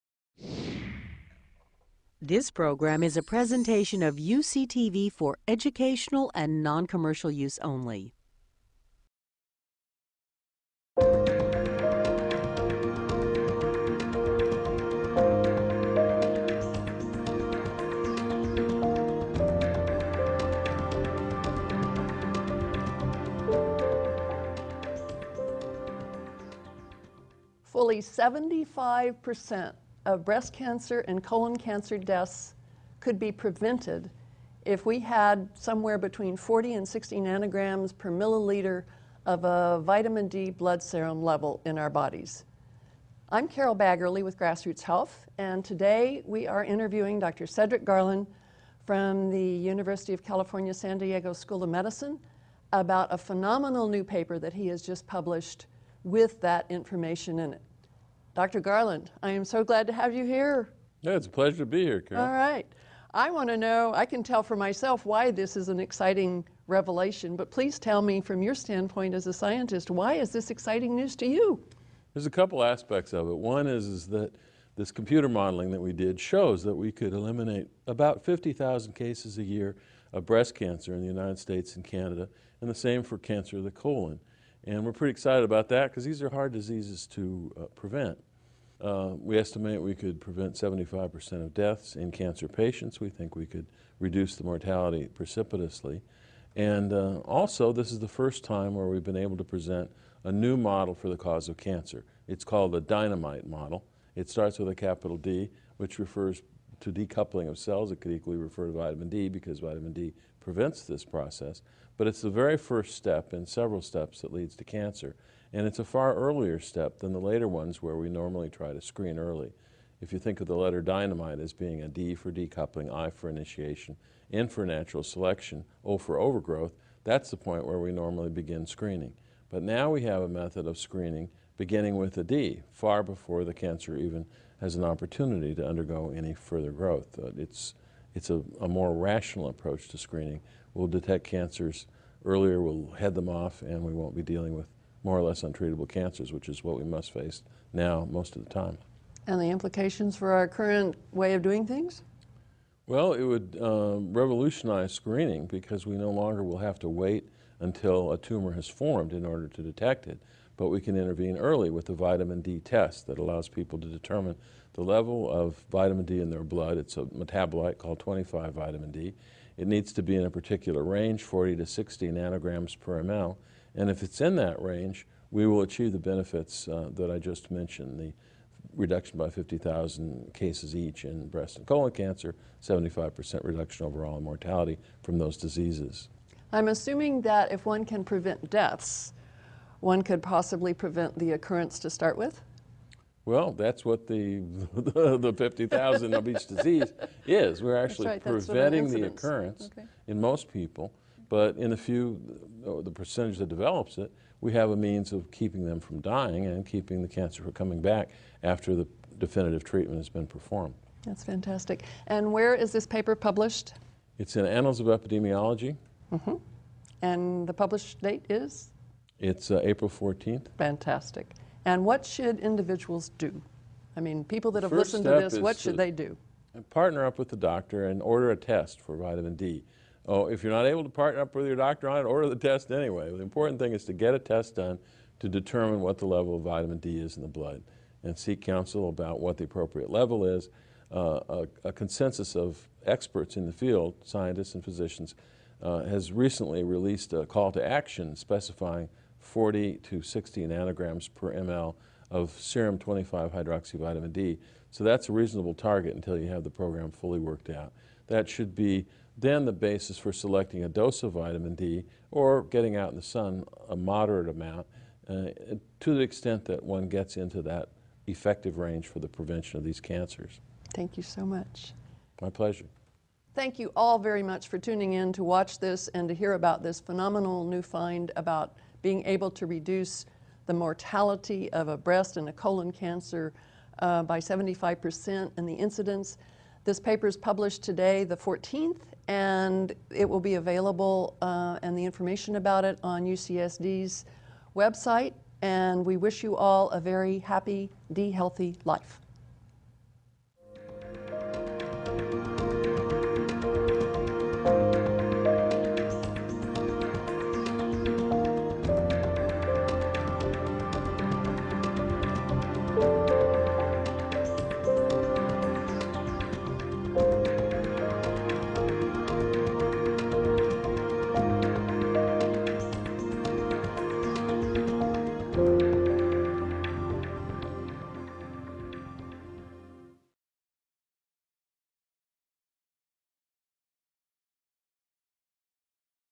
Possible 75% Less Cancer Deaths with Vitamin D
with Carole Baggerly and Dr. Cedric Garland

Transcription:
Announcer: This program is a presentation of UCTV for educational and non-commercial use only.

Carole Baggerly (B): Fully 75% of breast cancer and colon cancer deaths could be prevented if we had somewhere between 40 and 60 ng/ml of Vitamin D blood serum levels in our bodies. I'm Carole Baggerly with GrassrootsHealth and today we are interviewing Dr. Cedric Garland from the University of California, San Diego School of Medicine about a phenomenal new paper that he has just published with that information in it. Dr. Garland, I am so glad to have you here!

Dr. Cedric Garland (G): It's a pleasure to be here, Carole!

B: Alright! I want to know, I can tell for myself why this is an exciting revelation but please tell me from your standpoint as a scientist, why is this exciting news for you?

G: There's a couple aspects of it. One is that this computer modeling that we did shows that we could eliminate about 50,000 cases a year of breast cancer in the United States and Canada and the same for cancer of the colon. And we're pretty excited about that because these are hard diseases to prevent. We estimate we could prevent 75% of deaths in cancer patients. We think we could reduce the mortality precipitously. And also, this is the first time where we've been able to present a new model for the cause of cancer. It's called the Dinomite Model. It starts with a capital D which refers to decoupling of cells. It could equally refer to vitamin D because Vitamin D prevents this process. But it's the very first step in several steps that leads to cancer and it's a far earlier step than the later ones where we normally try to screen early. If you think of the first letter in Dinomite as being D for decoupling, I for initiation, N for natural selection, O for overgrowth, that's the point where we normally begin screening. But now we have a method of screening beginning with the D, far before the cancer even has the opportunity to undergo any further growth. It's a more rational approach to screening, will detect cancers earlier, will head them off and we won't be dealing with more or less untreatable cancers which is what we must face now most of the time.

B: Implications for our current way of doing things?

G: Well, it would revolutionize screening because we no longer will have to wait till a tumour is formed in order to detect it but we can intervene early with the Vitamin D test that allows people to determine the level of Vitamin D in their blood.  It's a metabolite called 25 Vitamin D. It needs to be in a particular range, 40-60 ng/ml, and if it's in that range we will achieve the benefits that I just mentioned, the reduction by 50,000 cases each in breast and colon cancer and the 75% reduction overall in mortality from those diseases.

B: I'm assuming that if one can prevent deaths one could possibly prevent the occurrence to start with?

G: Well, that's what the 50,000 of each disease is! We're actually preventing the occurrence in most people but in a few, the percentage that develops it, we have a means of keeping them from dying and keeping the cancer from coming back after the definitive treatment has been performed.

B: That's fantastic! And where is paper published?

G: It's in Annals of Epidemiology.

B: And the published date is?

G: Its April 14th.

B: Fantastic! And what should individuals do? I mean, people that have listened to this, what should they do?

G: The first step is partner up with a doctor and order a test for Vitamin D. If you're not able to partner up with your doctor on it, order the test anyway! The important thing is to get a test done to determine what the level of Vitamin D is in your blood and seek counsel about what the appropriate level is. A consensus of experts in the field, scientists and physicians, has recently released a call to action specifying 40-60 ng/ml of serum 25-hydroxy Vitamin D so that's a reasonable target until you have the program fully worked out. That should be then the basis for selecting a dose of Vitamin D or getting out in the sun a moderate amount to the extent that one gets into that effective range for the prevention of these cancers.

B: Thank you so much.

G: My pleasure

B: Thank you all very much for tuning in to watch this and to hear about this phenomenal new finding about being able to reduce the mortality of a breast and a colon cancer by 75% in the incidents. This paper is published today, the 14th, and it will be available and information about it on UCSD's website and we wish you all a very happy, D-healthy, life.